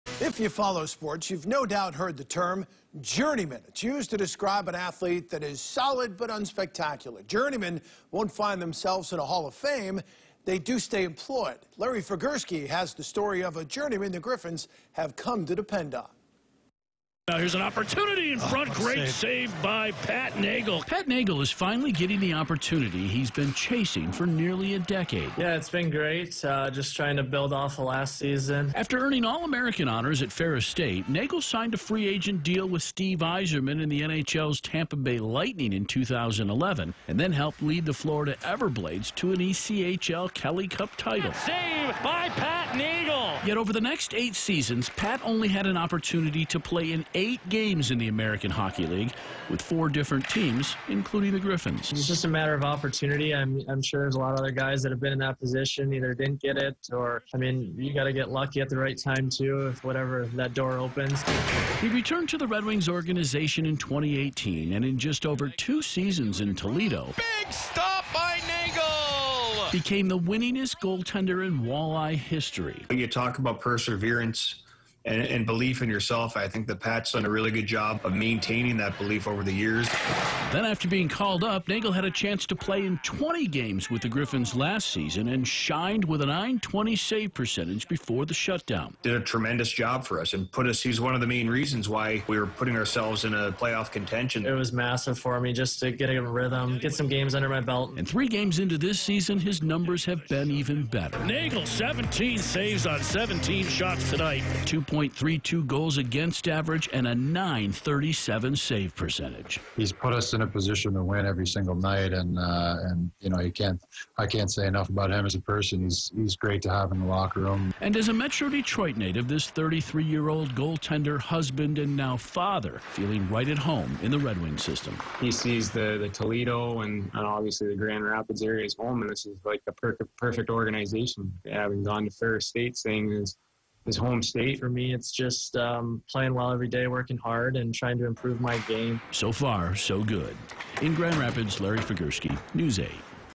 ‘A matter of opportunity’: Goalie Pat Nagle shines with Griffins
After chasing opportunities in minor league hockey for nearly a decade, goalie Pat Nagle made it to the Grand Rapids Griffins. The team soon found itself depending on him. (Feb. 24, 2021)